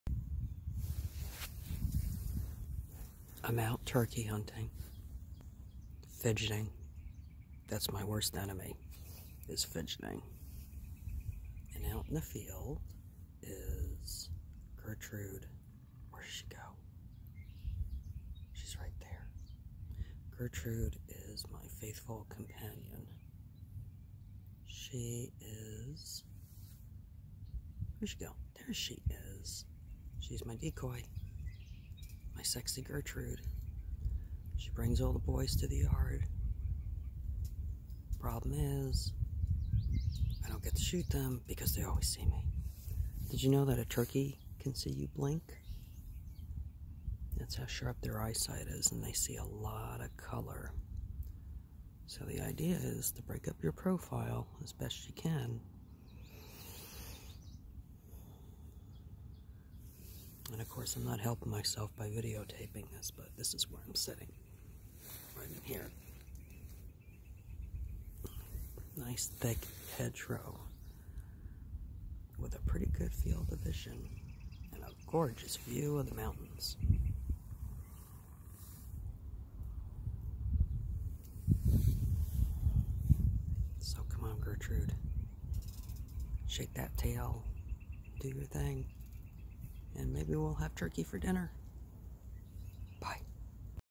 Figity Woman Turkey Hunter & Her Faithful Companion Gertrude!
It's May and spring turkey hunting in New York and I'm my worst enemy. My gear is top notch, my calls are perfect and I always see action but have yet to get a turkey home for dinner. Why? Because I can't sit still. I need to see everything and have trouble focusing.
Turkey survive by their excellent vision and hearing. Keep low, break up you profile and for the love of Pete, sit still!